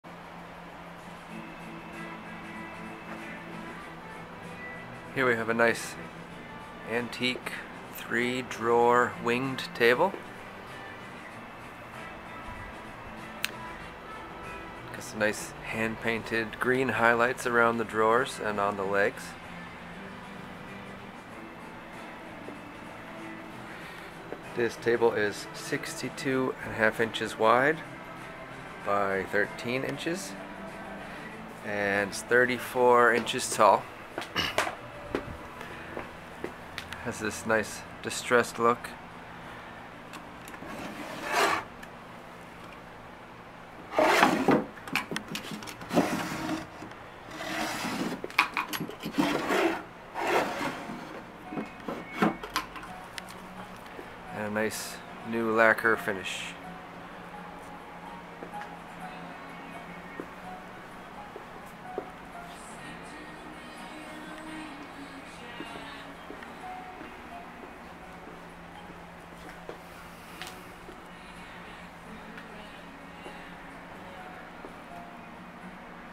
Oriental Furniture Warehouse-Tibetan Hand Painted Antique Floral Winged Three Drawer Table
Hello Friends!

See a first person view of this lovely product here.  Follow the link below to see more;

Our vintage Tibetan table is in original condition with forest green, naturally worn wood grain, and a carved apron and side wings. With its narrow depth of thirteen inches and length of sixty two inches, this carved oriental table is perfect for a hallway or a limited space. Consider a mirror, painting or screen above this antique for a dramatic look. This naturally distressed collectible antique table is about 60-90 years old and restored for modern use. The condition is excellent. Hand tooled and made of solid woods such as poplar, pine, and cedar. The painting is original, distressed, and enhanced by restoration for generations of use. Add this culturally painted antique to your interior design plans. These are rare, one of a kind, and priced at import direct cost.

Size 62" W x 13" D x 35"H
Vintage-60-80 Year old
Restored for modern use
No assembly required
Hand painted, hand made
Condition- Excelent
Slightly distressed, original color
Hand carved apron
Stretcher on legs for strength
Brass pulls
Solid mixed woods
Rare
Three Drawers.
Size inside drawer, 13" W x 9"D x 4"H

JOY IS HERE - TEAM ORIENTAL FURNISHINGS